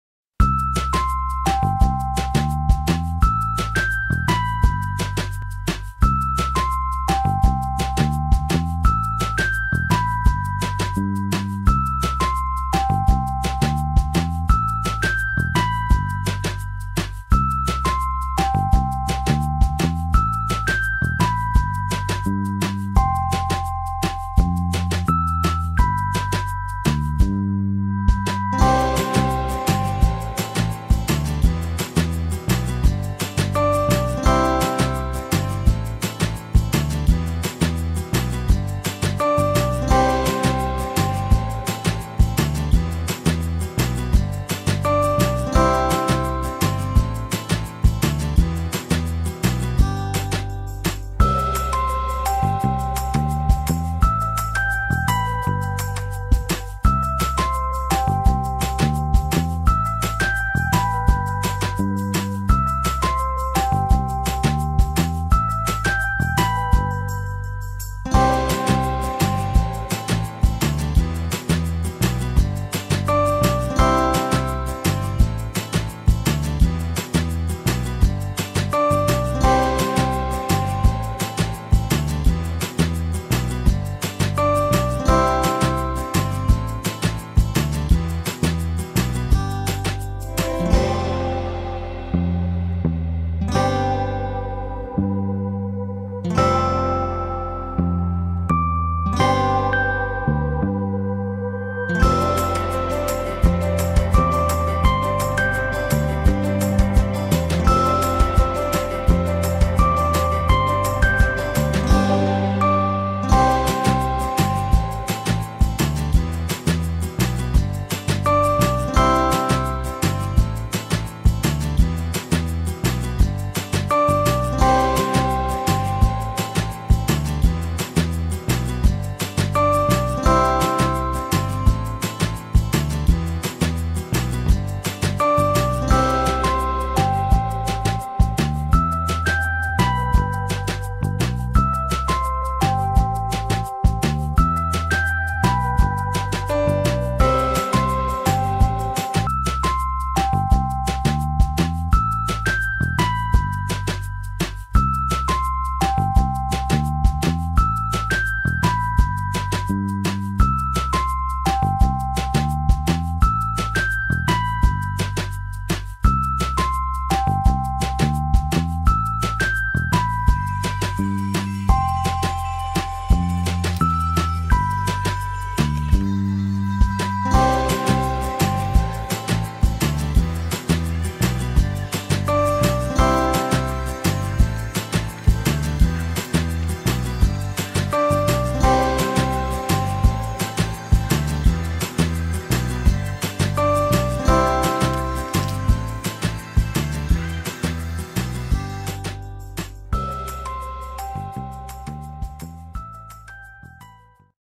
DIY Bridal Shower Tea Light Favors
These are some tea light favors I made for my sister's bridal "tea party" shower.